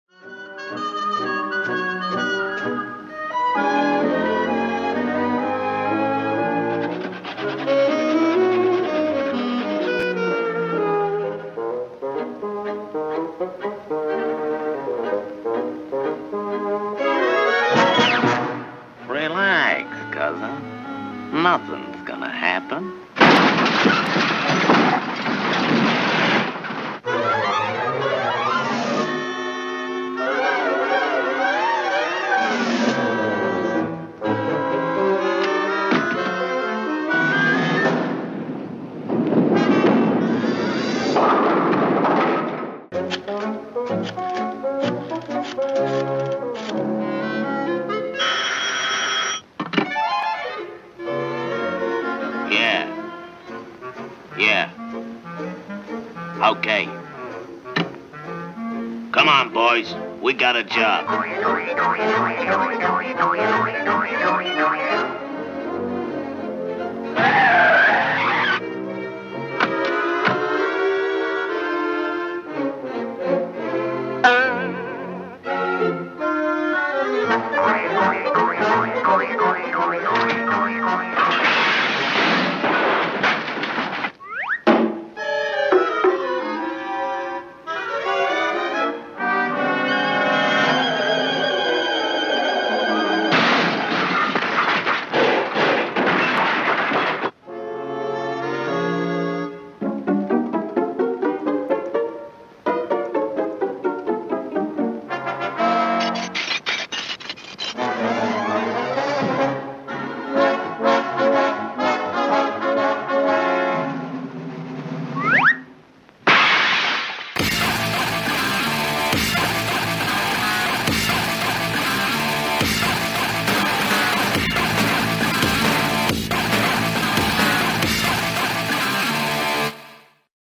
The real reason why NESTS was defeated (a KOF parody cartoon)
Based on Tom & Jerry episode 57: Jerry's cousin.
Beware of Seirah's brother.